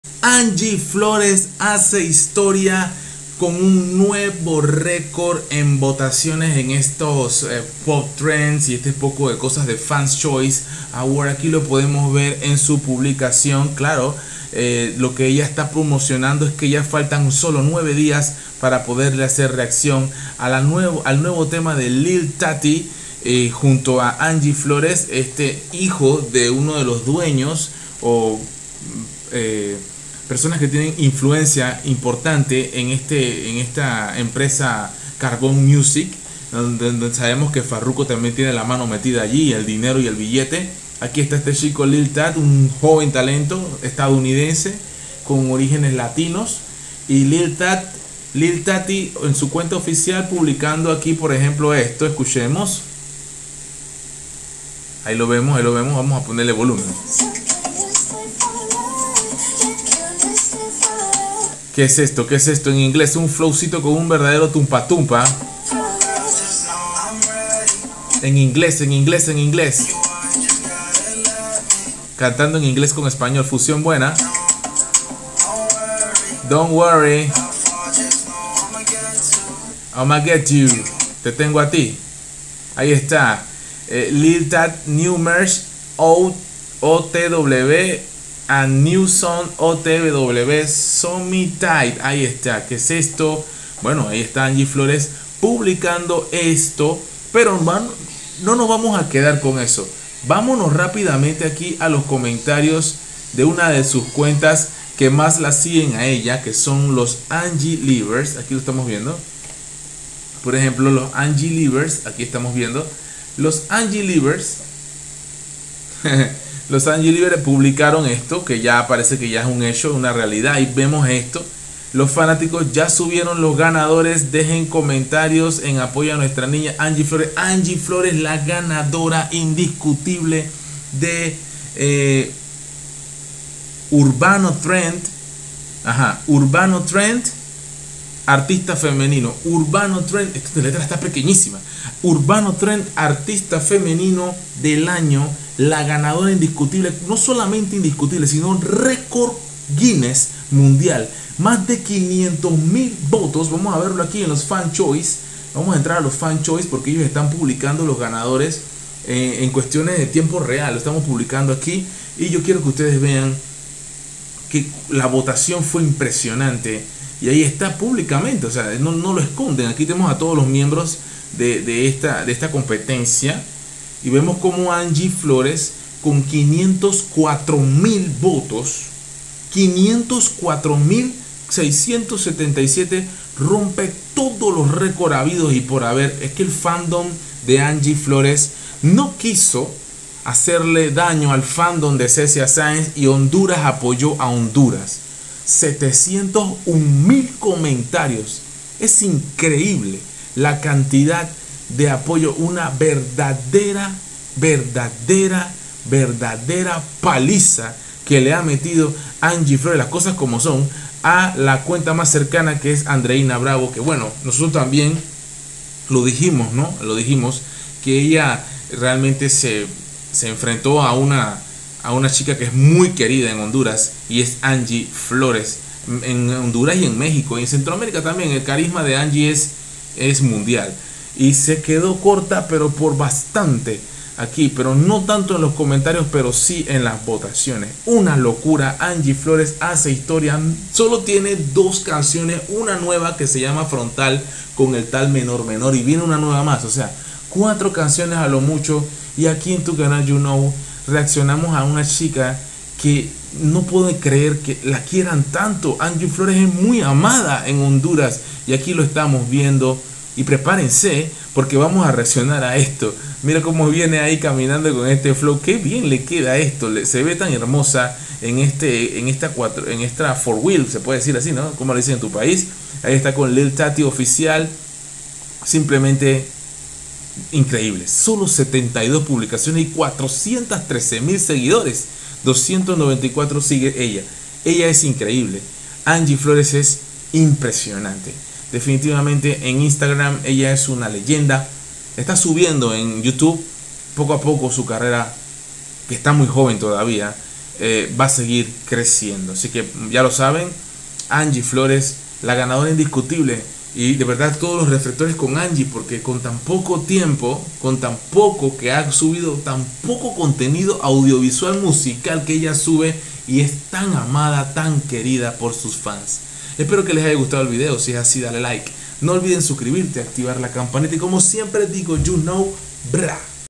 ANGIE FLORES 🇭🇳 ROMPE TODOS LOS RÉCORDS en FANS CHOICE AWARD 2023✅ y Superó Fácil a ANDREINA BRAVO
NO TE PIERDAS MIS ÚLTIMAS REACCIONES 😱

reacciones, gana dinero por internet
PAGINAS EN FACEBOOK

QUIERES UN INGRESO EXTRA?💸